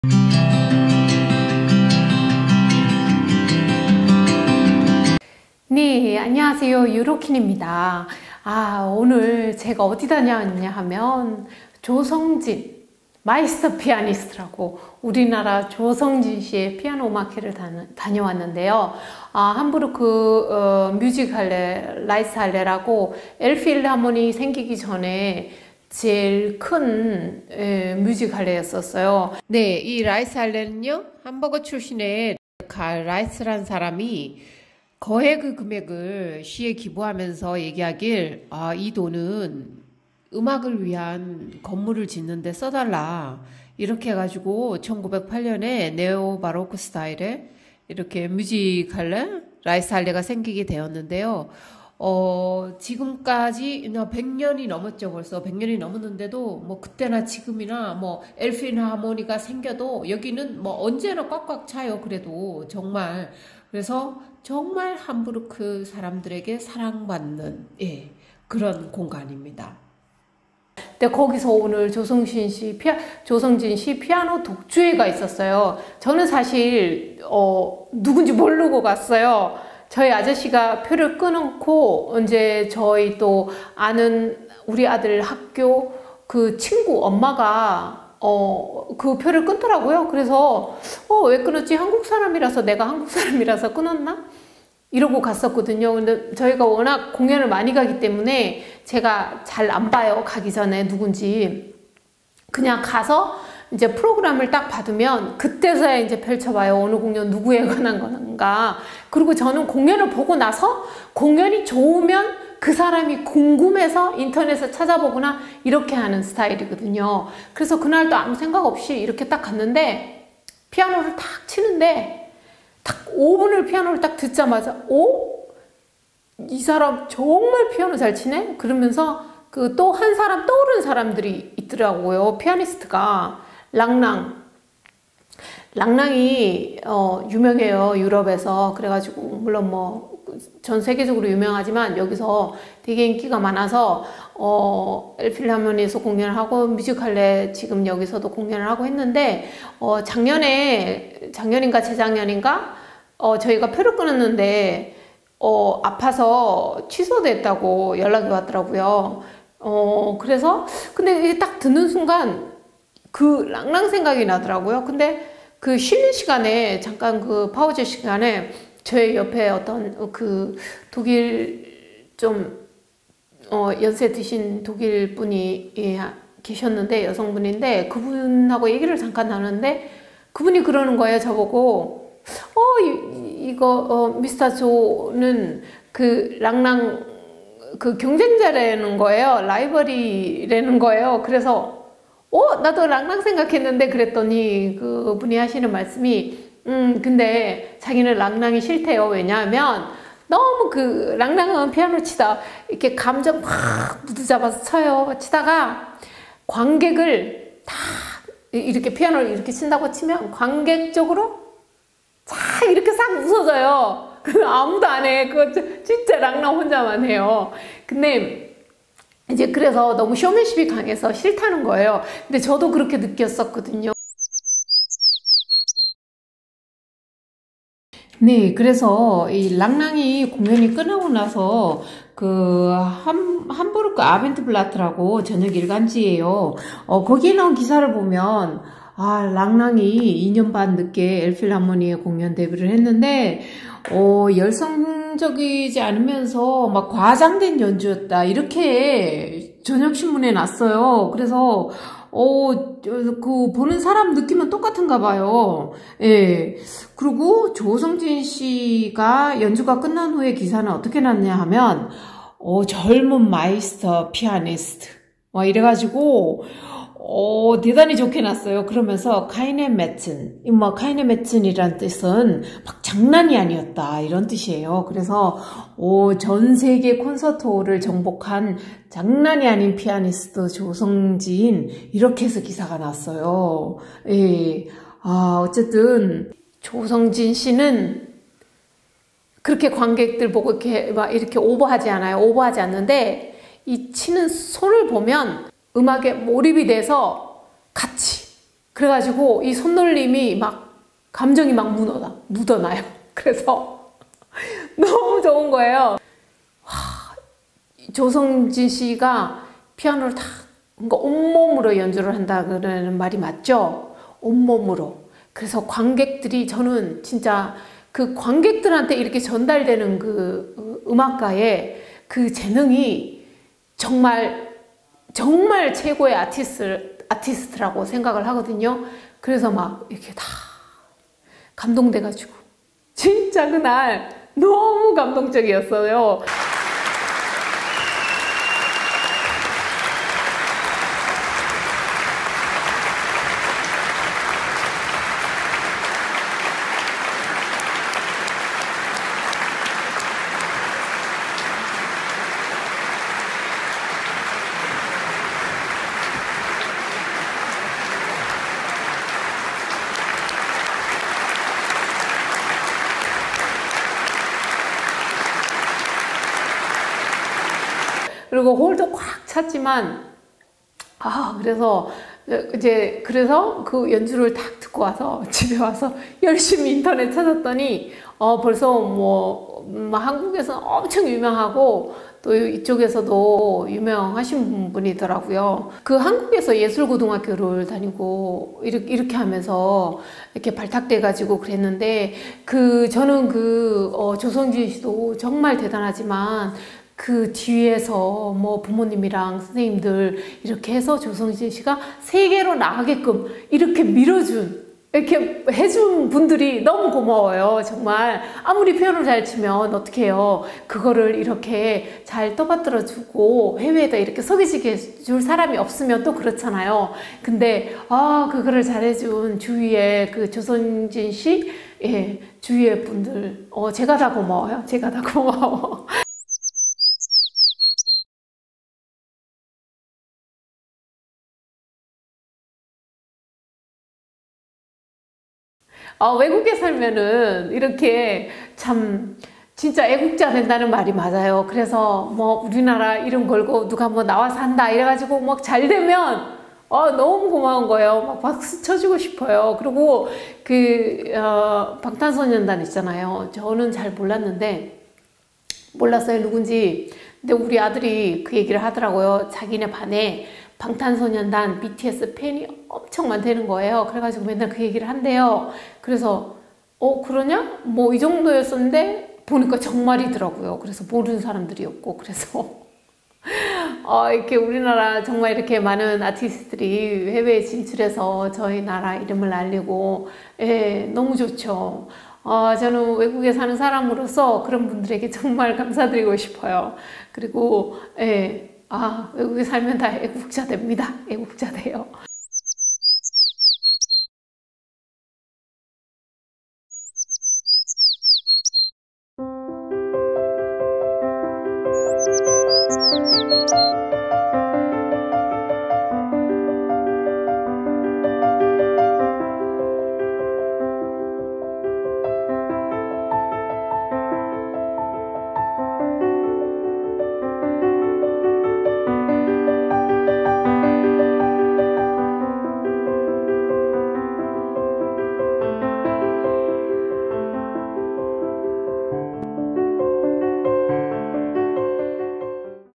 유로퀸 Euroqueen TV 조성진 피아노독주 함부르크 연주, 함부르크가 극찬한 피아니스트 Seong-Jin Cho, Piano Concert, Laeiszhalle
안녕하세요 유로퀸입니다.
오늘 제가 우리나라 조성진 씨의 피아노 음악회를 다녀왔는데요.
함부르크  Laeiszhalle(또는 뮤직할레라고도 함) 라고 Elbphilharmonie 생기기 전에 함부르크에서 메인콘서트홀이었었구요.
지금도 엘필하모니에 손색없이 나란히 중요한 콘서트들을 개최하고 있읍니다.
Hello everyone, this is Euroqueen.
Today I visited a piano concert held by Meisterpianist Seong-Jin Cho at Music Hall Laeiszhalle in Hamburg.